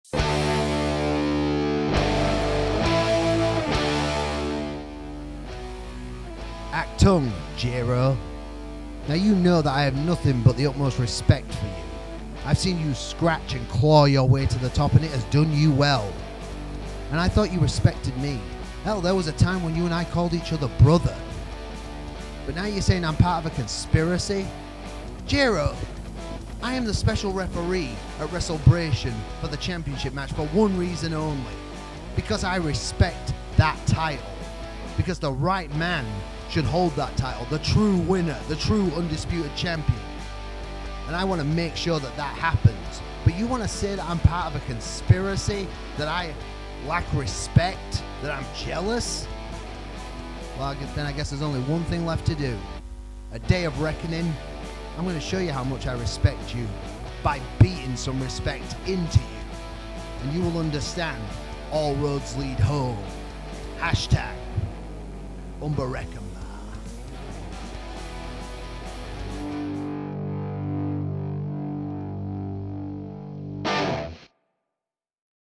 Tommy Dillon Responds to Jayrow's Comments: Day of Reckoning 26-06-21
UKW Day of Reckoning Live from the UKW Arena, Saturday, 26 June, 2021